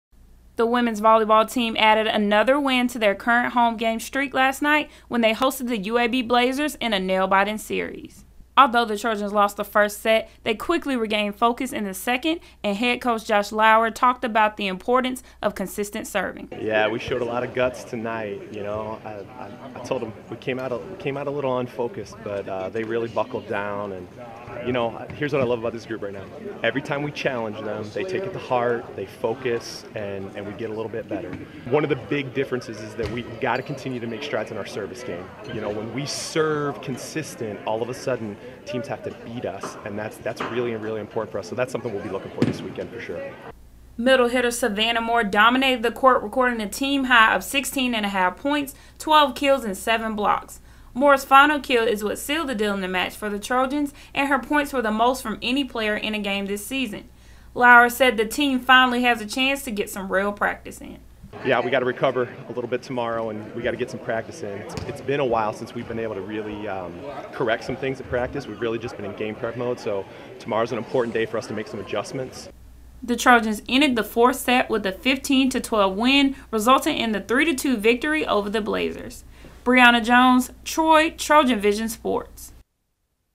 TROY Volleyball vs UAB - TROY TrojanVision News
The volleyball team is now three and five after winning a thriller against the UAB Blazers in Trojan Arena Tuesday night. TROY TrojanVision News’ Briana Jones has the highlights.